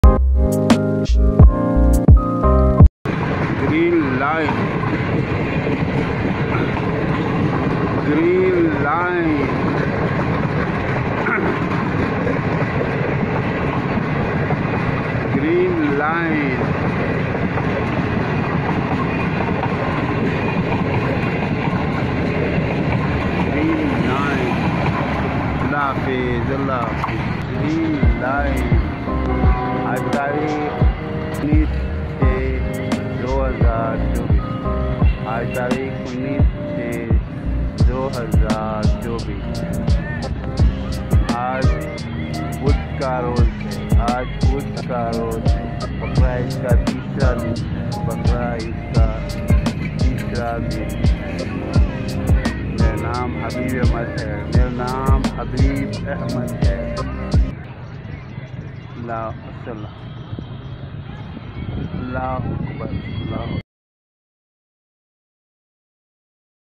Green line express train Ac standard travel vlog
Thanks ❤️

Full Information video of Green line nice train Islamabad to karachi|Ac standard|economy class
_______**"your searching"**________